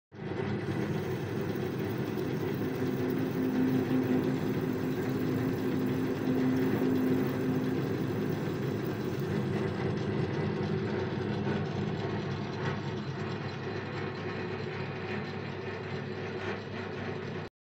Boxford Model A, 2mm (0.080") autofed cut in carbon steel
Boxford Model A bench top lathe with 0.5Hp motor performing a 2mm (80 thou) cut in carbon steel (car driveshaft)
Machine running at 650rpm at 0.072mm/rpm feed rate.